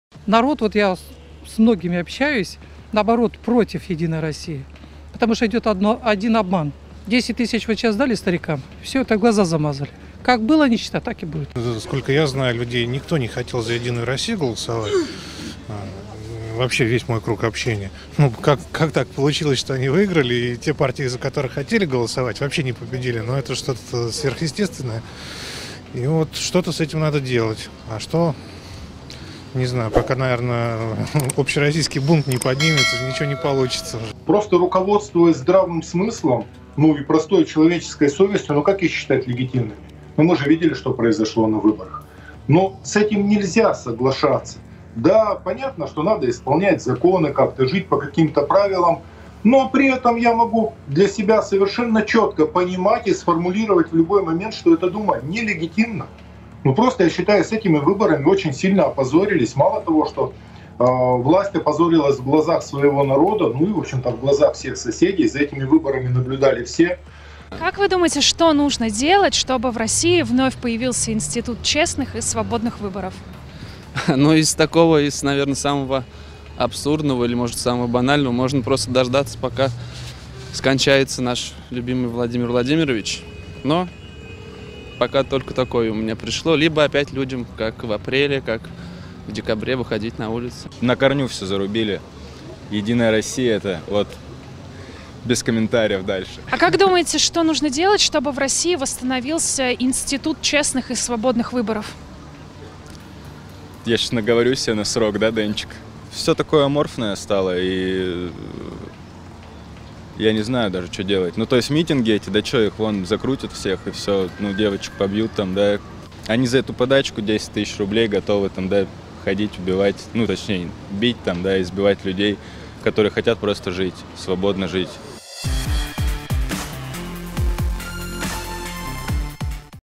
«С этим нельзя соглашаться»
Элла Памфилова пообещала судиться с журналистами, которые якобы распространяют «ложь и клевету» о выборах в Госдуму: «Все сейчас отмониторим, отследим. Я думаю, что таких исков [против журналистов] будет немало. Раньше мы этого не делали. И зря».

Об исках против фальсификаторов выборов г-жа Памфилова, естественно, даже не заикнулась. Да и когда у нас реально наказывали тех, кто помогает жуликам и ворам оставаться у власти?

Напомню: согласно исследованию аналитика Сергея Шпилькина, который уже 10 лет изучает выборную статистику, единороссы приписали себе 14 млн «аномальных» голосов, то есть 50% голосов за «Единую Россию» могли быть сфальсифицированы.